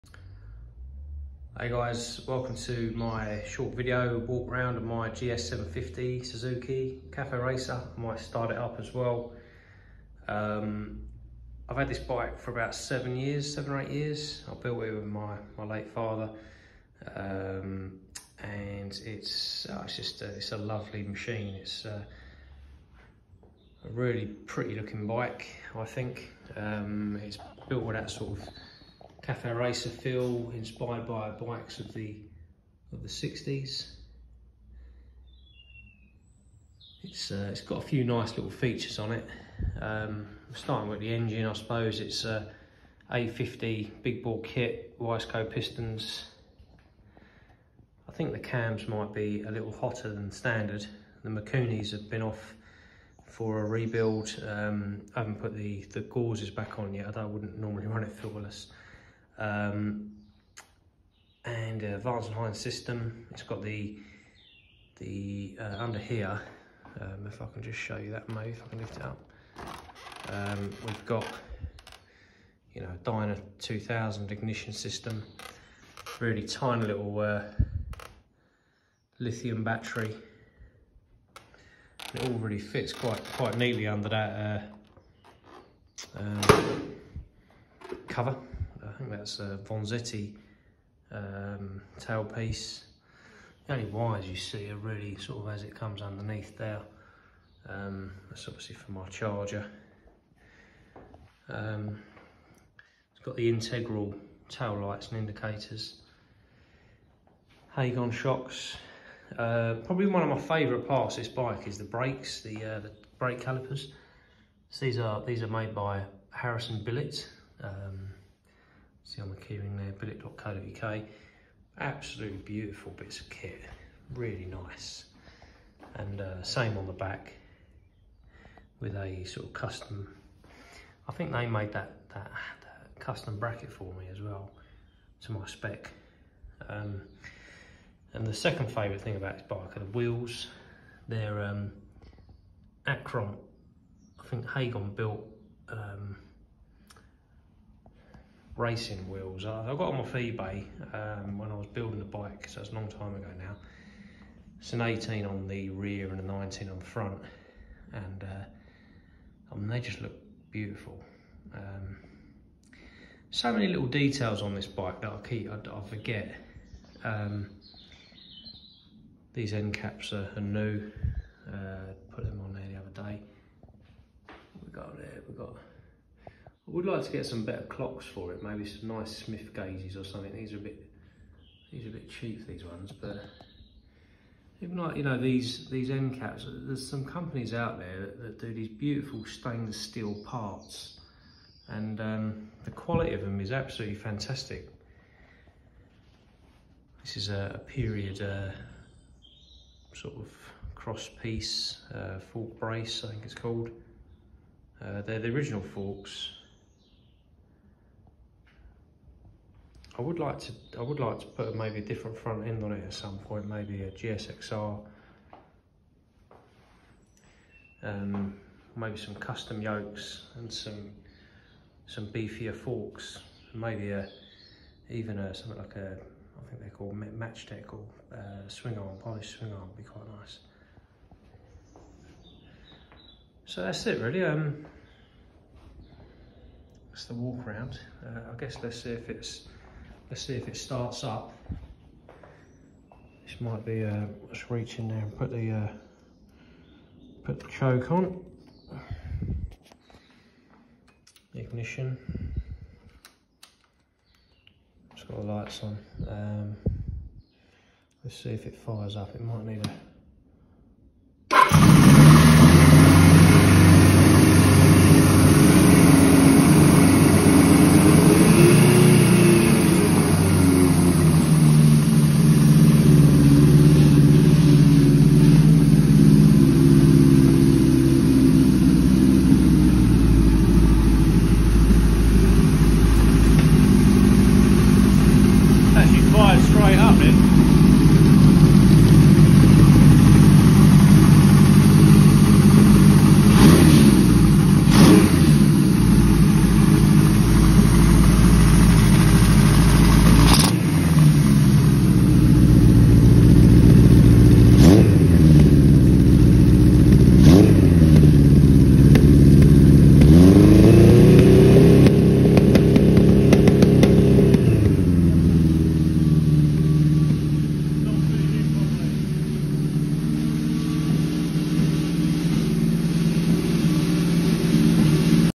Suzuki GS750 cafe racer walk around and start up
The build video of this bike that I uploaded has over 75,000 views, also on my channel.  See the finished bike and hear her purr!